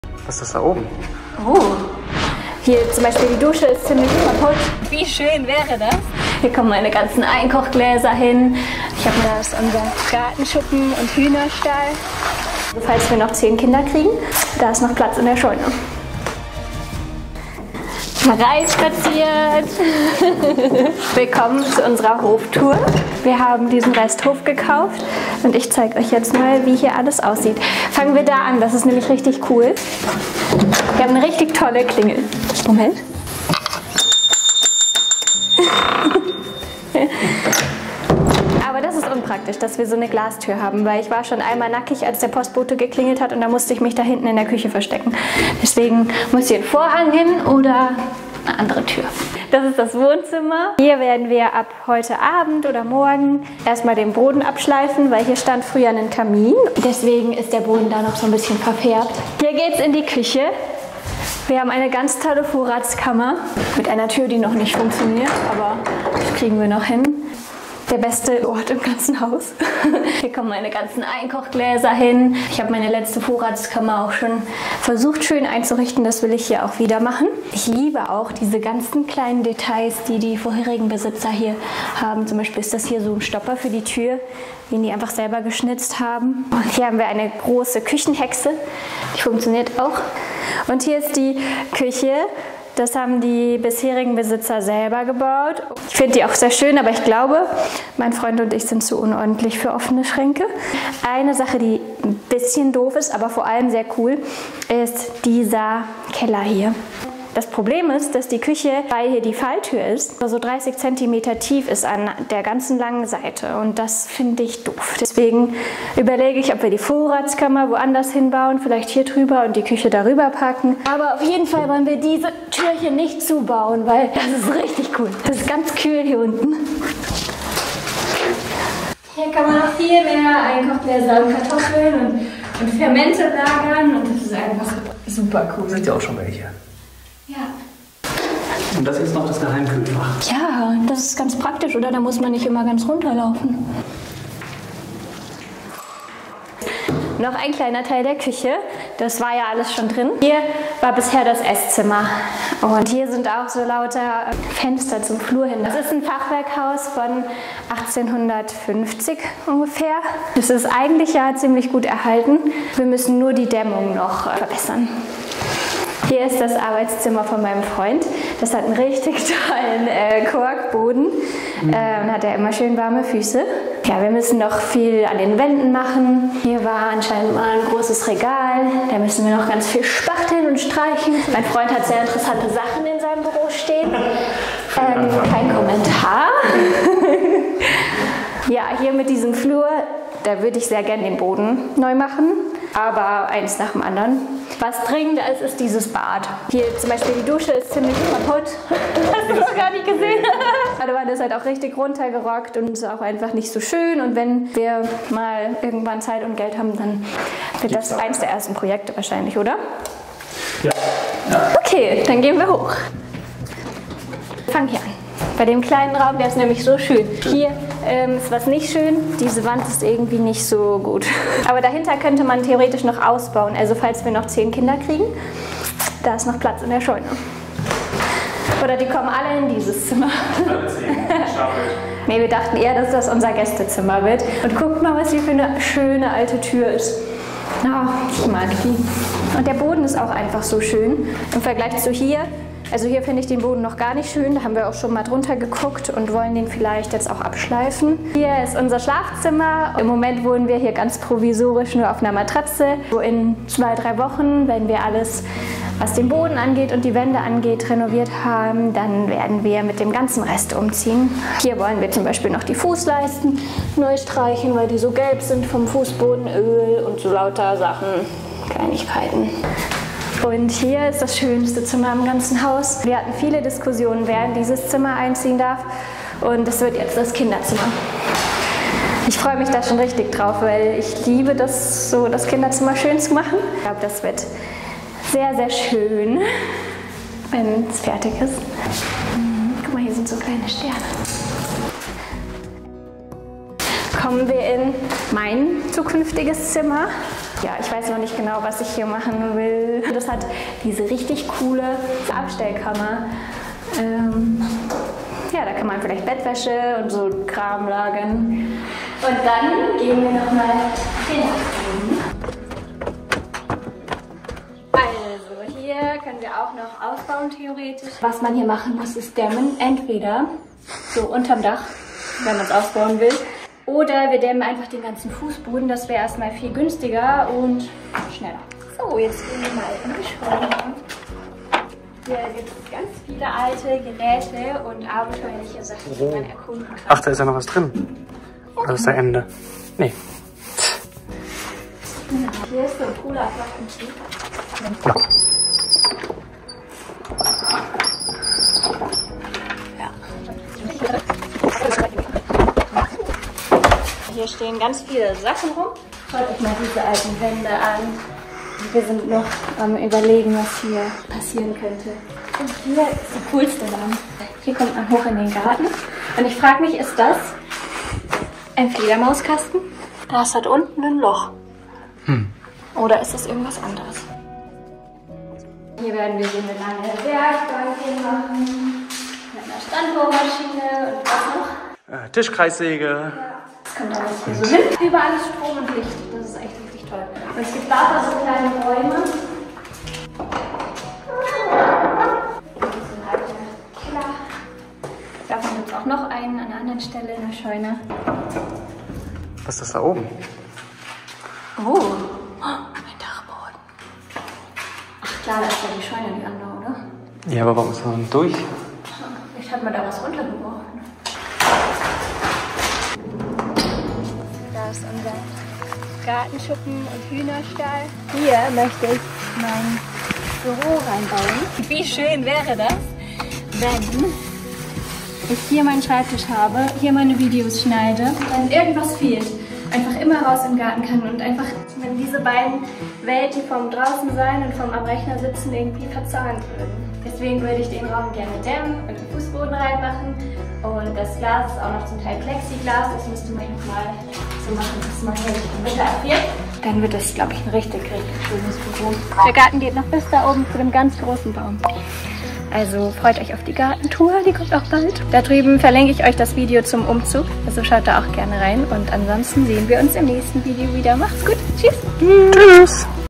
HOF TOUR: Das ist unser neues Zuhause!
Wir kommen in unserem neuen Zuhause immer mehr an – und entdecken nach und nach jeden Winkel. Ich liebe, dass dieser alte Bauernhof so viel Charakter ausstrahlt! 🤗 Baujahr 1850, ist das zu glauben?!Jeden Tag stoßen wir auf neue Überraschungen! Bei einem so alten Haus verbergen sich aber auch wirklich hinter jeder Türe kleine Rätsel, mysteriöse alte Gerätschaften und Ecken, die ein bisschen Liebe gut vertragen könnten.Deshalb: Höchste Zeit für eine kleine Hausführung! Kommst Du mit auf Entdeckungstour? In meinem neuen Video nehme ich Dich mit!